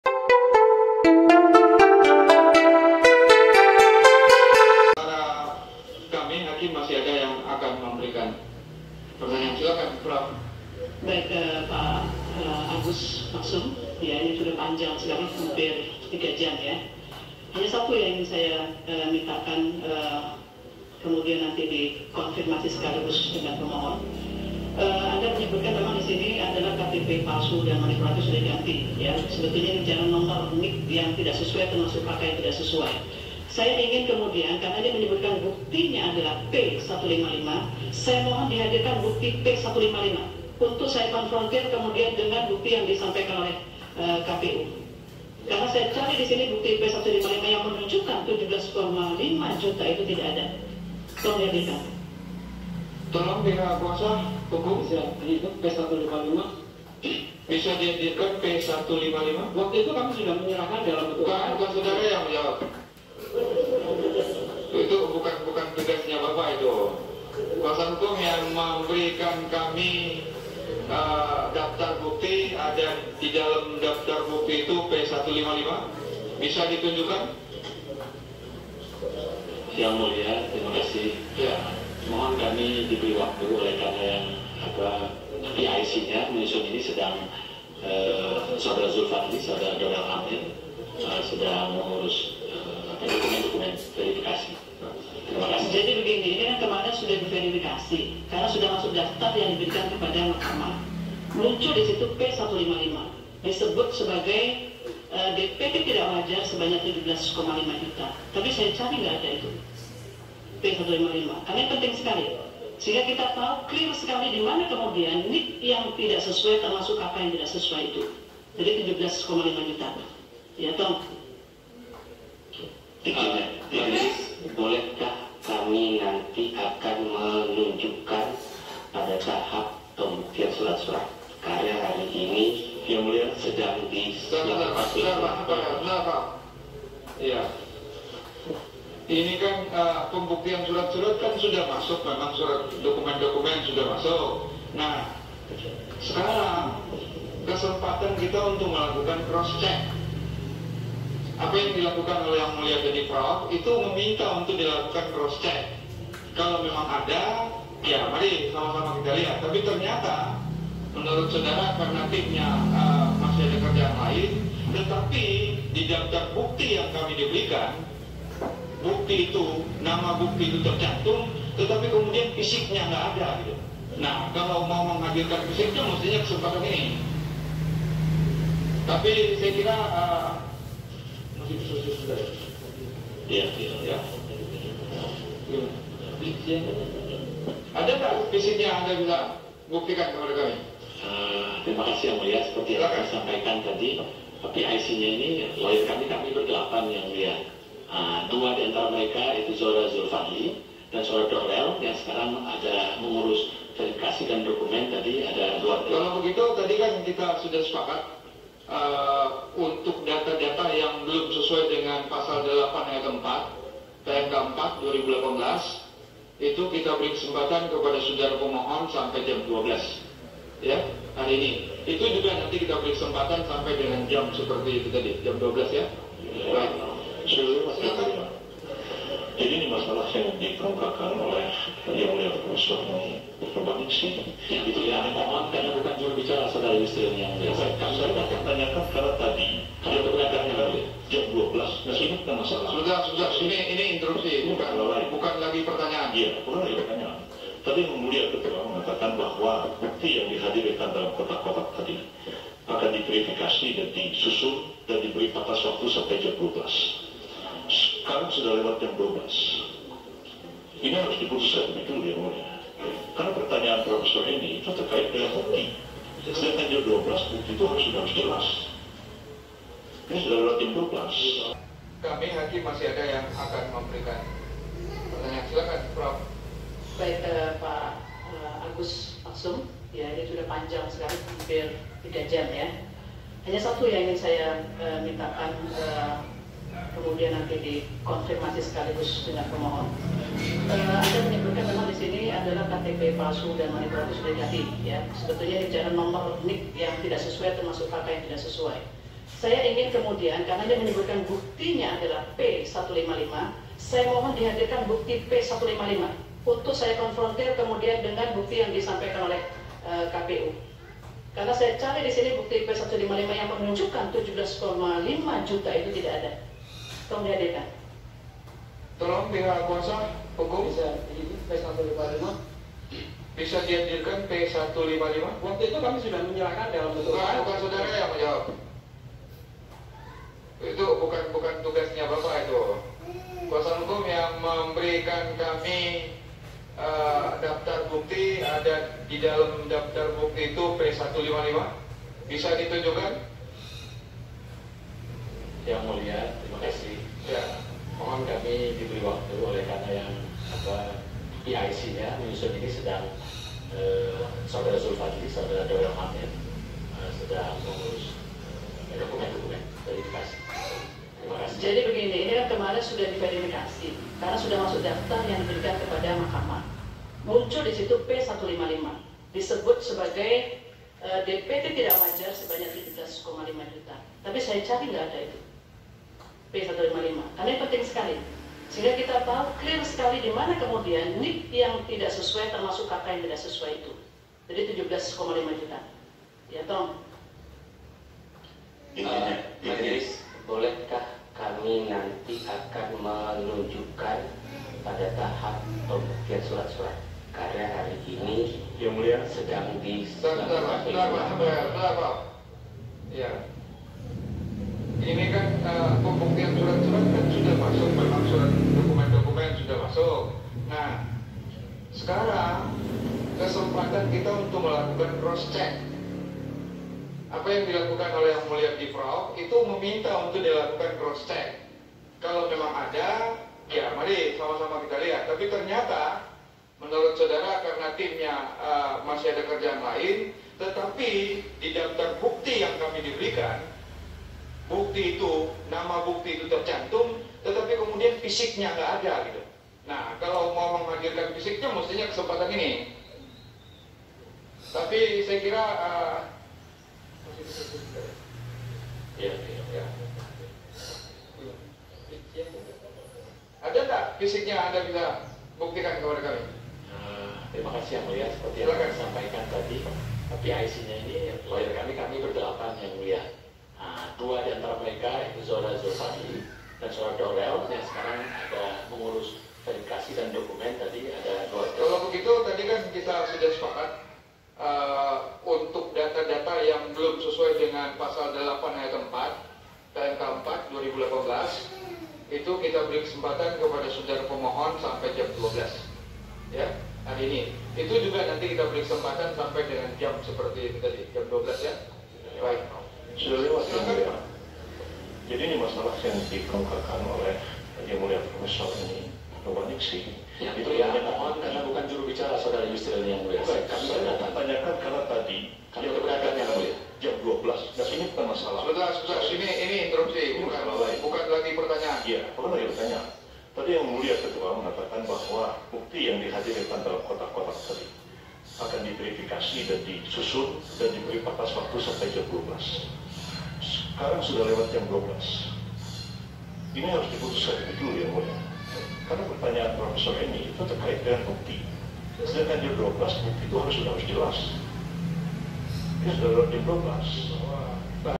Barang bukti P.155 dicari pada sidang MK 19 Juni 2019
Kuasa Hukum 02 Minta Waktu Saat Hakim MK Minta Bukti 17,5 juta DPT Tidak Wajar, Yusril Interupsi

Rabu, 19 Juni 2019 13:10
Berawal dari keterangan saksi bahwa "Ada 17,5 juta NIK palsu, di mana tanggal lahir yang tidak wajar," ujar Agus Muhammad Maksum.
Menurut Agus Muhammad Maksum, dari 17,5 juta DPT, terdapat 9,8 juta pemilih yang tanggal lahirnya sama, yakni pada 1 Juli.
Kemudian, ada 5,3 juta yang lahir pada 31 Desember. Selain itu, ada 2,3 juta yang lahir pada 1 Januari.
"Itu tidak wajar, karena yang lahir 1 Juli itu ada 20 kali lipat dari data normal," kata Agus Muhammad Maksum.

Hakim MK Enny Nurbaningsih mengatakan bahwa dalam gugatannya Tim Kuasa Hukum Prabowo-Sandi menuliskan bahwa P155 menjadi bukti tuduhan 17,5 juga DPT tidak wajar.
Suasana sidang lanjutan Perselisihan Hasil Pemilihan Umum (PHPU) Pilpres 2019 di gedung Mahkamah Konstitusi, Jakarta, Selasa (18/6/2019). Sidang tersebut beragendakan mendengarkan jawaban termohon, pihak terkait dan Bawaslu.
Meski ditulis dalam permohonan gugatan, namun Hakim MK Enny Nurbaningsih tidak menemukan bukti P155 tersebut.

"Ini kan sudah diverifikasi karena sudah masuk daftar yang diberikan muncul disitu P155 disebut sebagai bukti DPT tidak wajar 17,5 juta tidak wajar,
Saya cari tidak ada itu karena itu penting sekali, karena clear sekali kemudian NIK yang tidak sesuai termasuk KK yang tidak sesuai itu," kata Hakim MK Enny Nurbaningsih
Tim Kuasa Hukum BPN Prabowo-Sandi Denny Indrayana lantas kembali meminta waktu untuk menunjukkan bukti P155 tersebut.

"Majelis boleh kah kami menunjukkan pada saat pembuktian surat-surat karena hari ini sedang dilakukan penyusunan," sahut Denny Indrayana.
Hakim MK Aswanto menjelaskan bahwa semua surat dan dokumen sudah diverifikasi.
Saat ini menurut Aswanto merupakan waktu yang tepat untuk Tim Hukum BPN Prabowo-Sandi melakukan cross check terhadap bukti yang dimiliki
"Ini kan pembuktian, surat-surat kan sudah masuk nah sekarang kesempatan kita untuk cross check apa yang dilakukan yang mulia untuk dilakukan cross check kalau memang ada ya mari sama-sama kita lihat, tapi ternyata karena timnya masih ada kerjaan lain tetapi kan di daftar bukti yang kami terima bukti itu tercantum tetapi fisiknya gak ada, kalau mau menghadirkan fisiknya pada kesempatan ini, ada gak fisiknya anda buktikan pada kami ?" kata Aswanto